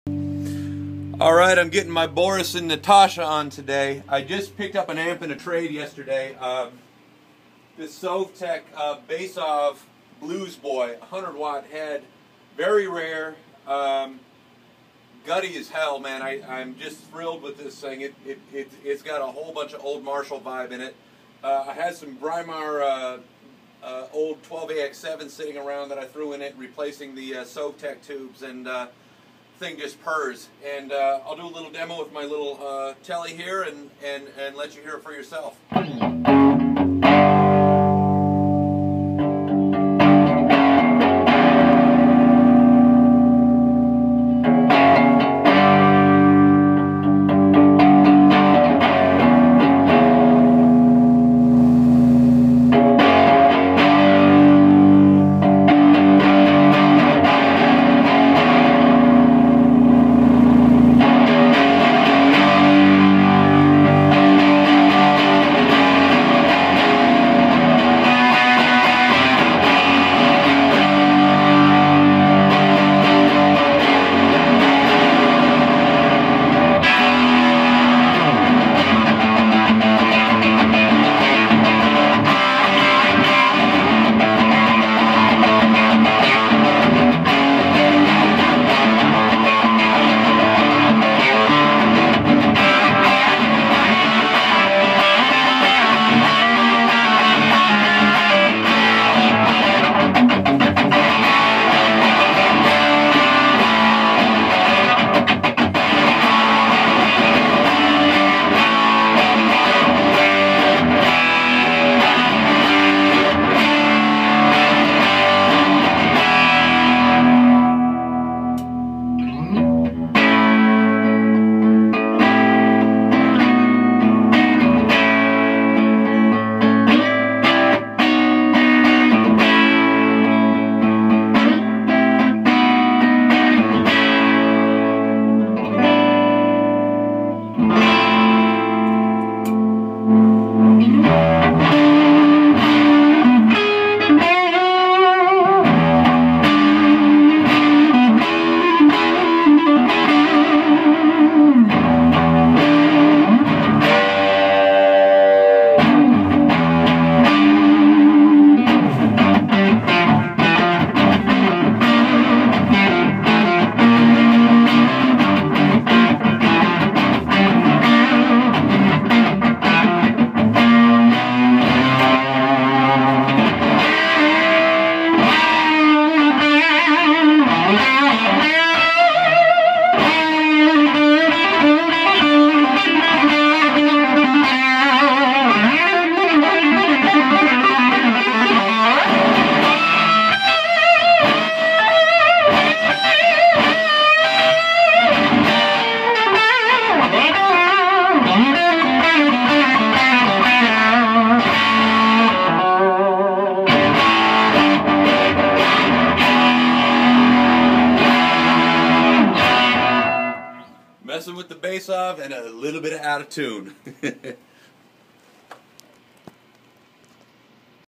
Sovtek Bassov Blues Boy 100 amp - Thinline Telecaster
Got this monster in a trade and am thrilled. An extremely rare Bassov Blues Boy 100, which to me sounds like a cross between my old Stramp, and Sunn Model T. Massive bottom end, warm, thick grind, and tons of sustain. Had a couple Brimar pre amp tubes sitting around and WOW. Running into an old West Cab loaded with a squareback Eminence and a late 70s G12-70 Celestion..